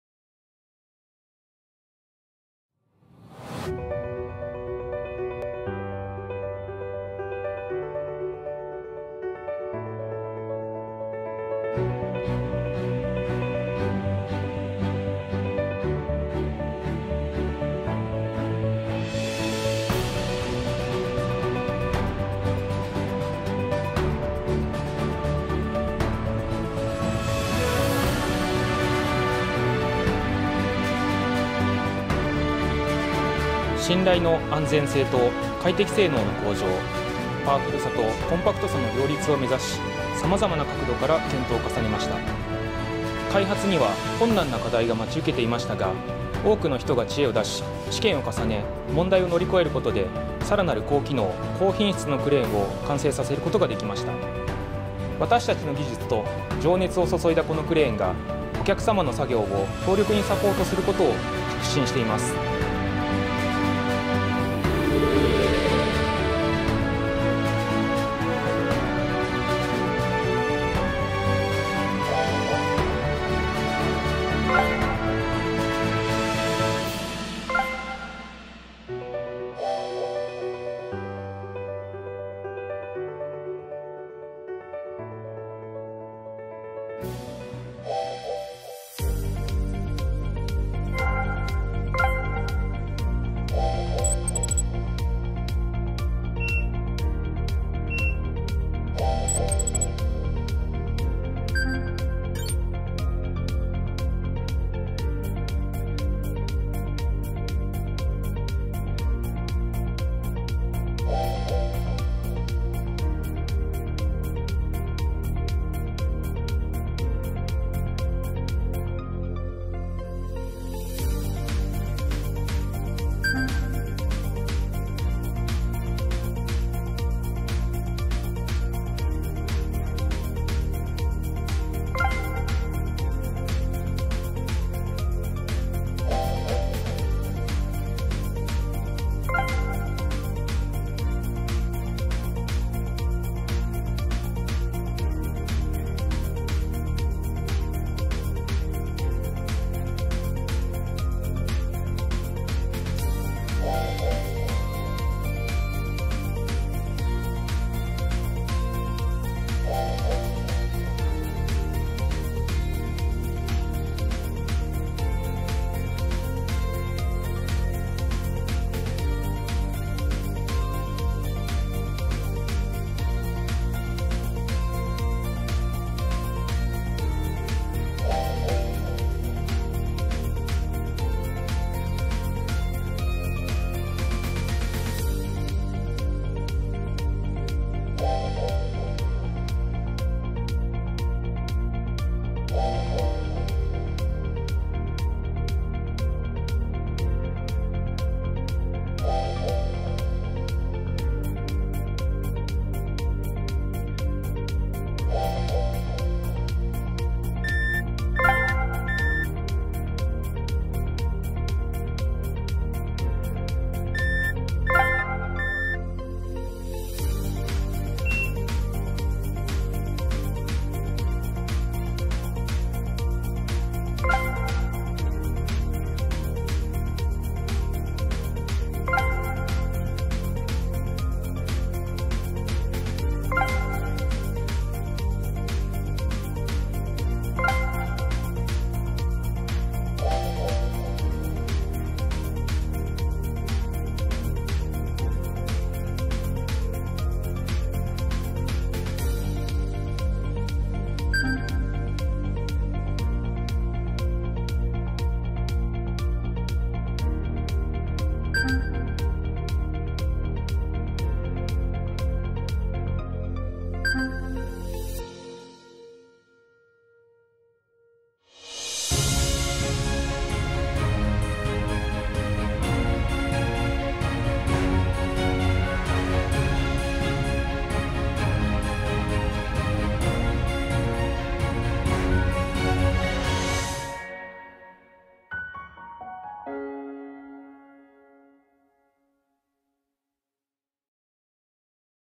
Maeda CC1908S-1 - Amazing Mini Crawler Crane
The CC1908 model boasts many market-leading qualities including a massive 8.1-ton lifting capacity, a 20.1m lifting height and 3.5-ton pick and carry capacity. Features of this model include a touchscreen 10.4″ monitor display, boom slew limit setting, 360° surround-view camera and monitor, wide view roof window and a sunshade and visor as standard. No outriggers mean an extremely slim working footprint and this model has a class-leading short tail swing and slew radius for work in restricted areas.

In addition, this environmentally-friendly machine is equipped with a Yanmar Diesel engine that is EU Stage V and EPA Final Tier 4 compliant making it more fuel-efficient. Other highlights of this crane include a fully enclosed air-conditioned cab with link slide door, powerful two speed hi/lo winch, hydrostatic transmission, 6/4/2 fall hook block, tilt sensor/alarm, anti slack device and automatic heptagonal 5-section boom. Optional features are a 1.5-ton Hydraulic Fly Jib, a new 2-ton Searcher Hook and Blade.

8.1 ton Max Capacity
20.1m Max Working Height (26m with Optional Fly Jib Attachment)
3500kg Pick & Carry Duty
10.4-inch Multi-Monitor
Powerful Two Speed Winch with Hydraulic Disc Brake
360 degrees slew angle
Slew Restriction Device
Hydrostatic Transmission
6 Fall / 4 Fall / 2 Fall Hook Block
Tilt Sensor/Alarm
360° Surround-View Camera
Automatic Pentagonal 5-Section Boom
Data Logger
2 Year Warranty